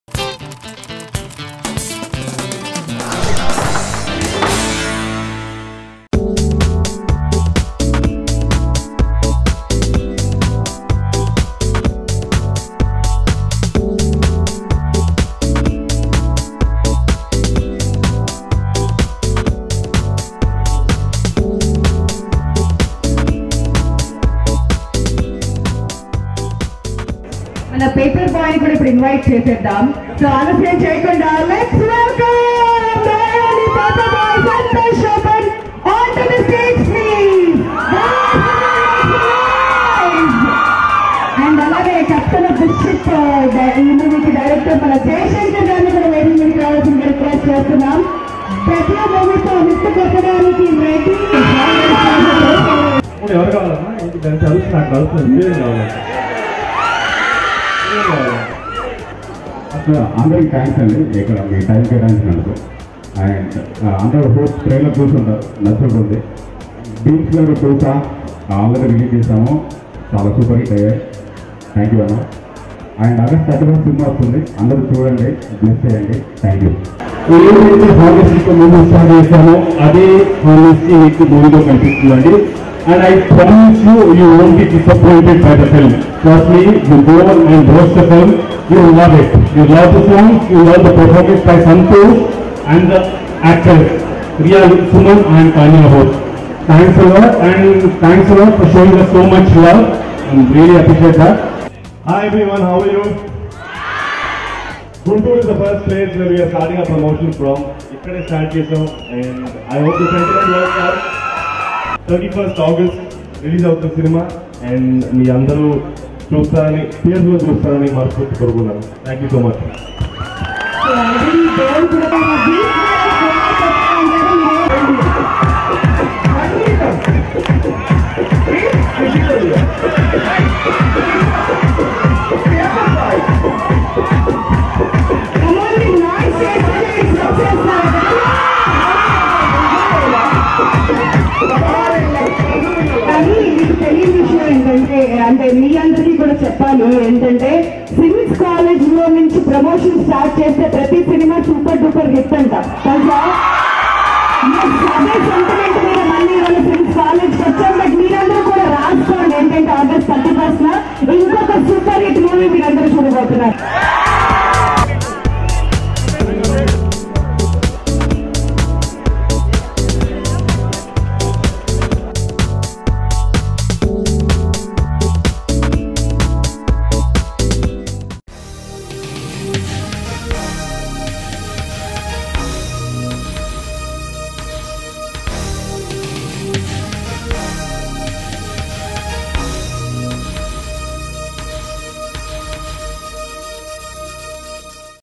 Paper Boy Promotions in GUNTUR | Paper Boy Team ROAD TRIP | Sampath Nandi | Santosh Shoban | Tanya
Paper Boy Promotions in GUNTUR at Paper Boy Team ROAD TRIP on Mango Telugu Cinema. PaperBoy 2018 Telugu Movie ft. Santosh Shoban, Riya Suman, and Tanya Hope. PaperBoy is Directed by Jaya Shankar and Music composed by Bheems. Produced by Sampath Nandi under the banner Sampath Nandi Team Works.

Happy Watching!